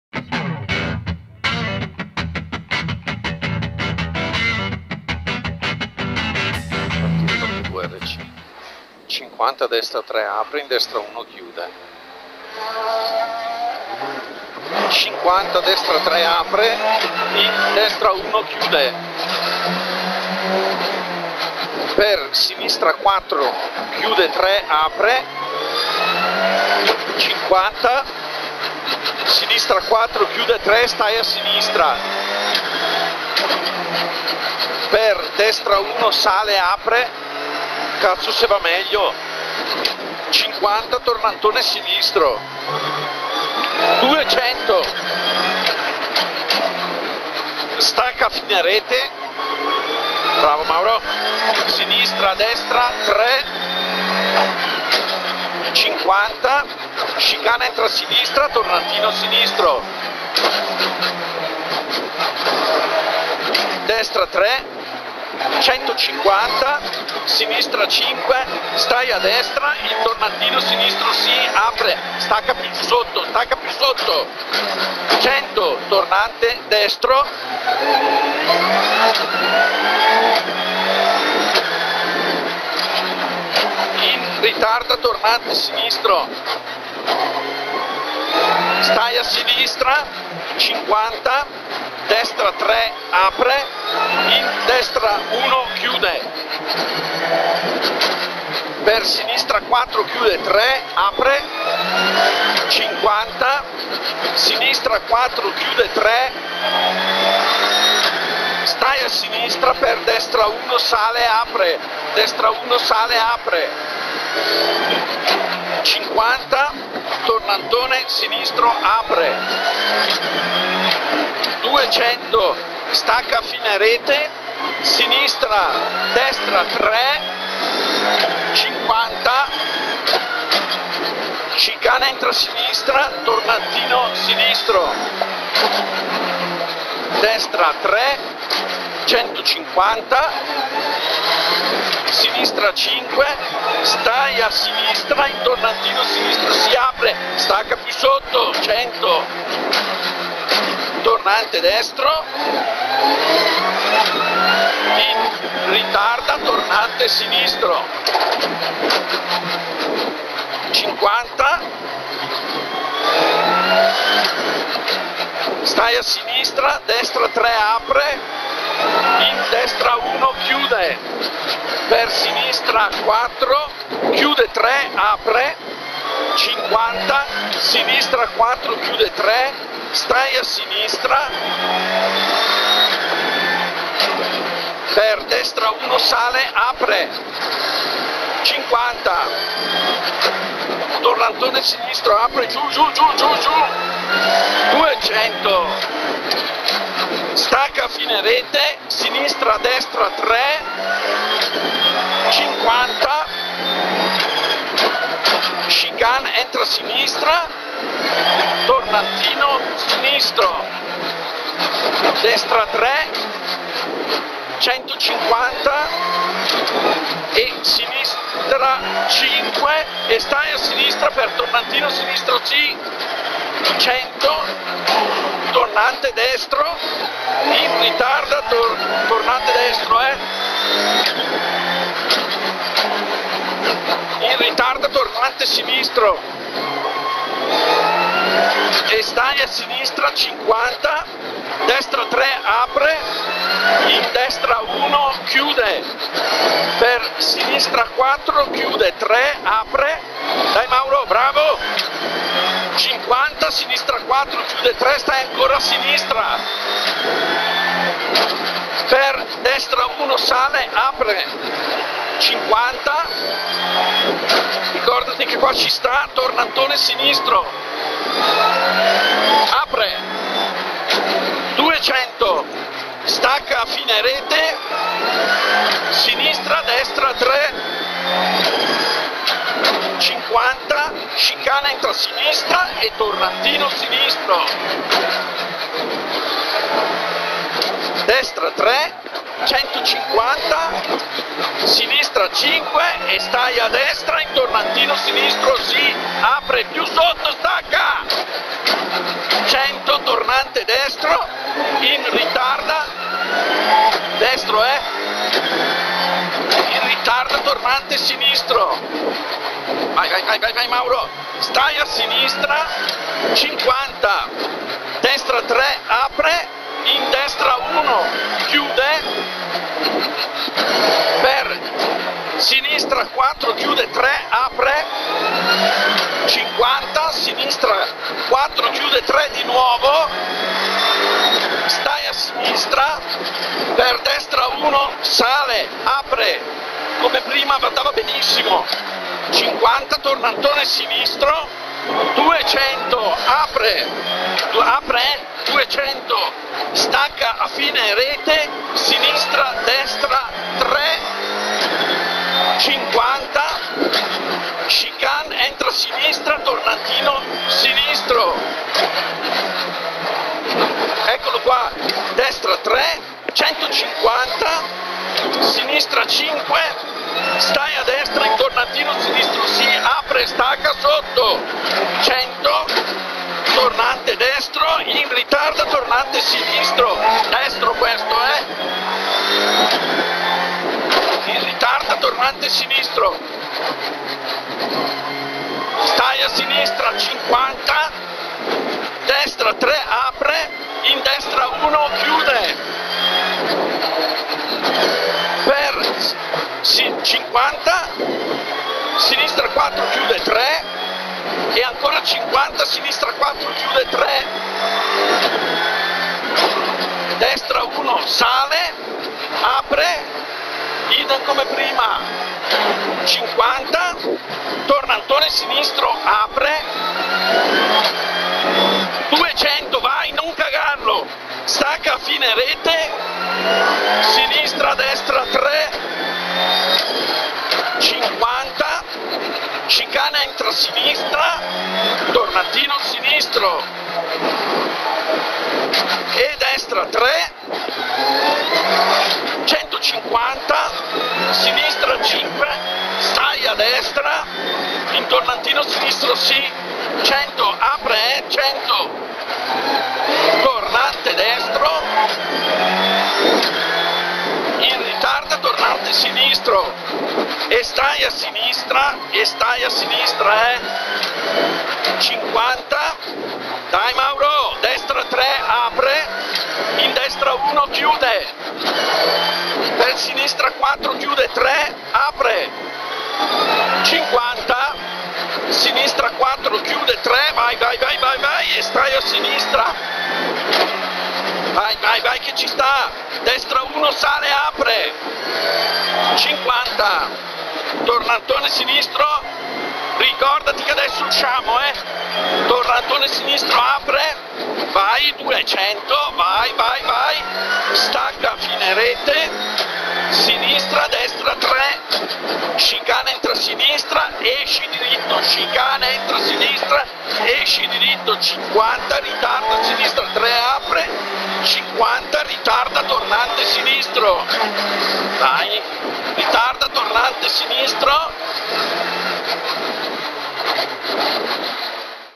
7°MRS Pavia 2023 OBC INVERNICI-ZANELLA ps 5 by Ferrario
Onboard con l'equipaggio INVERNICI-ZANELLA su Citroen C3 R5 durante la ps 5 del 7°MRS Pavia 2023.